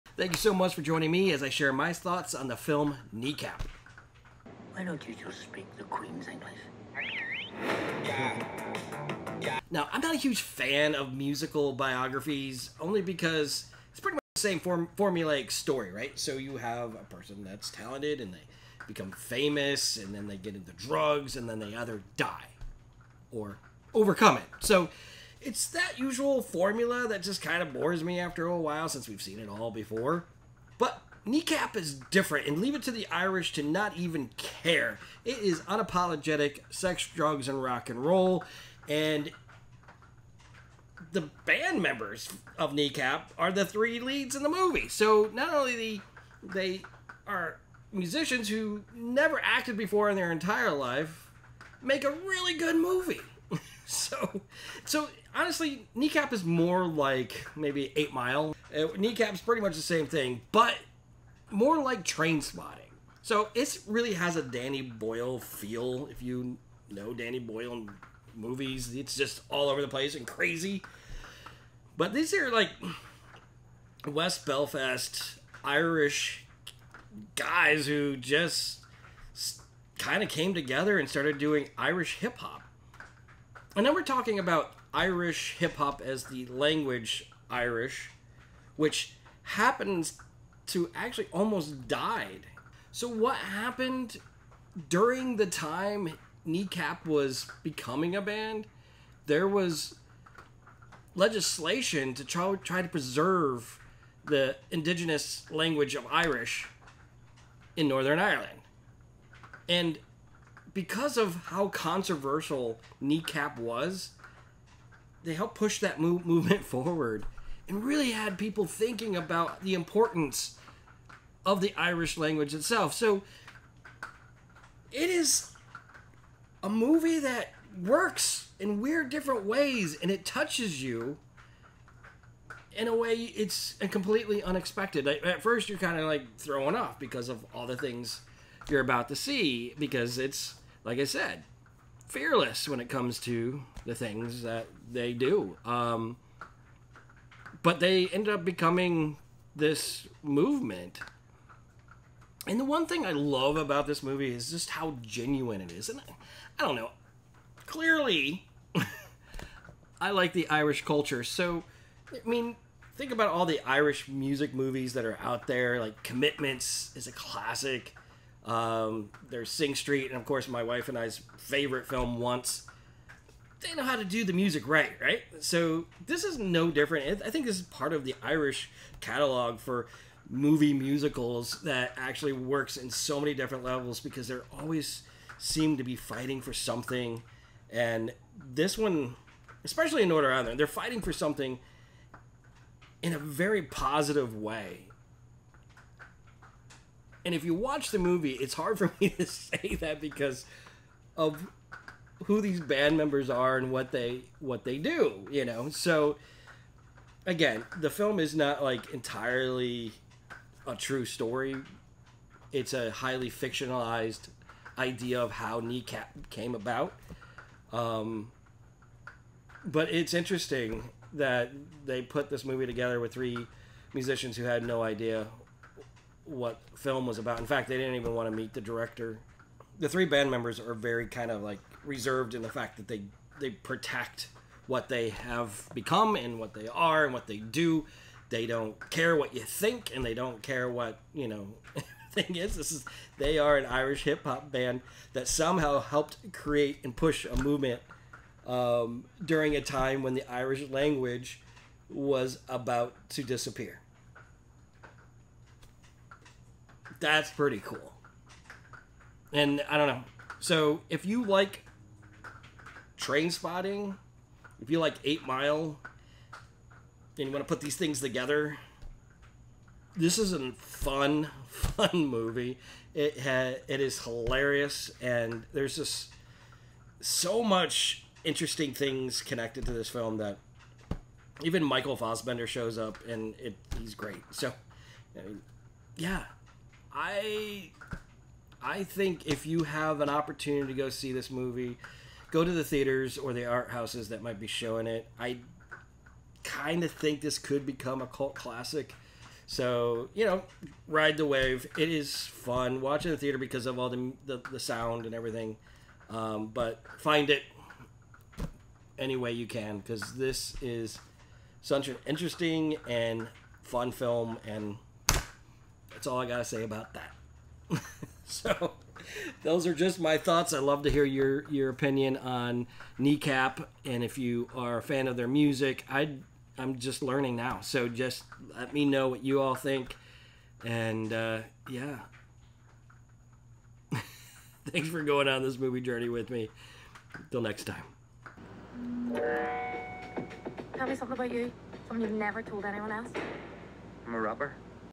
Kneecap (2024) Review: A Hilarious Irish Hip-Hop Gem and Cult Classic? (Full Movie Review) 🇮🇪
Is Kneecap destined for cult classic status? This Irish hip-hop comedy tells the story of a band sparking a social movement to save their language!  We dive deep into the film's outrageous humor, infectious music (all in Irish!), and surprisingly poignant message.  Will Kneecap blow you away or leave you saying "Eh"? Watch our full spoiler-free review to find out!

Shot: iPhone 15 Pro
Edited: Premiere Pro
Graphic: Adobe Express